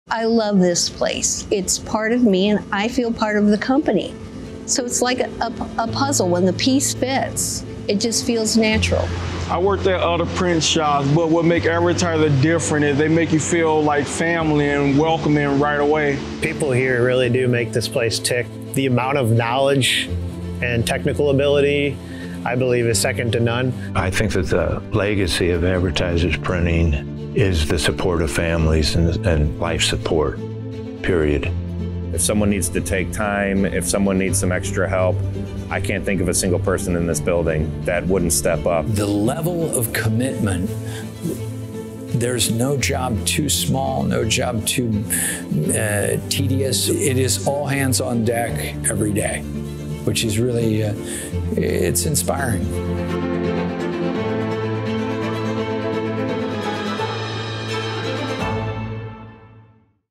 Family, Teamwork and Work Ethic | Advertisers Printing's secret to success
In this video, we highlight the significance of our team members and touch on what it is like to be a part of our family.